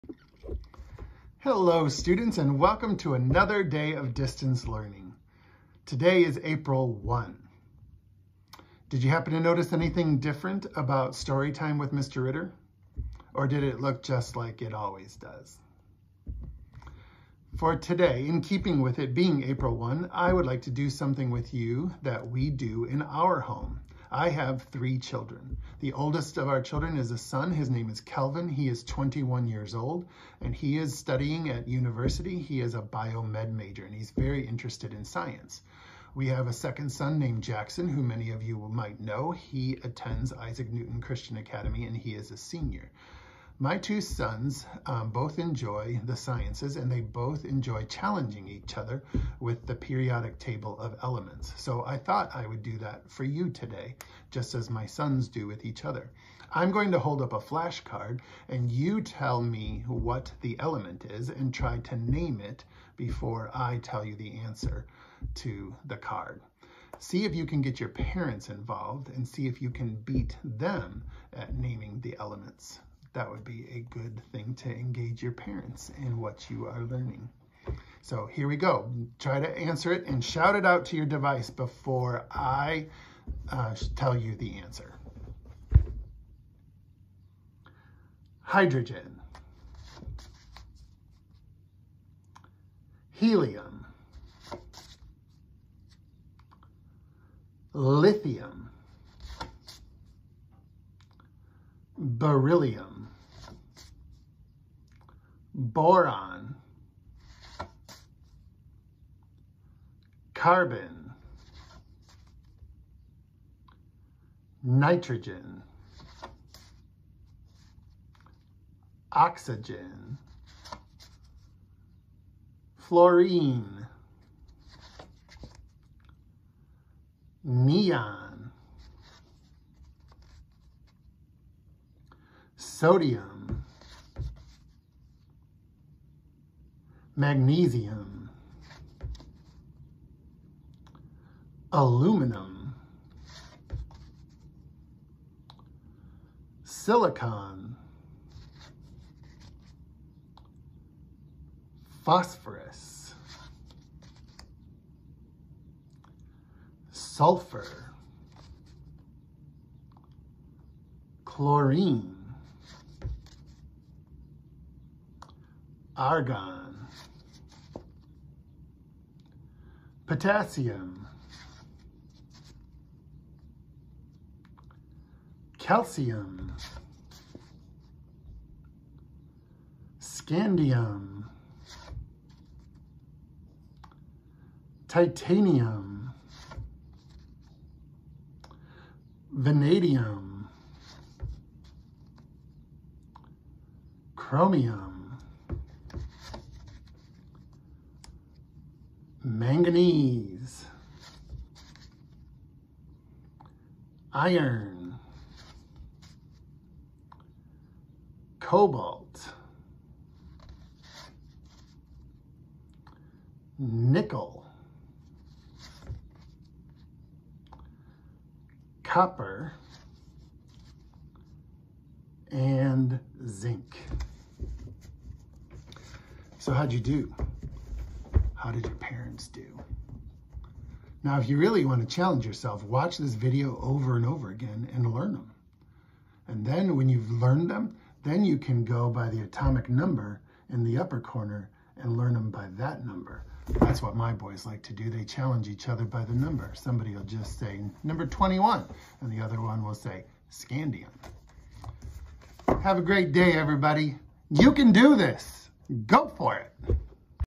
Periodic Table of Elements
Flash Cards With Mr. Ridder, Episode 1.  Periodic Table of Elements, Atomic Numbers 1-30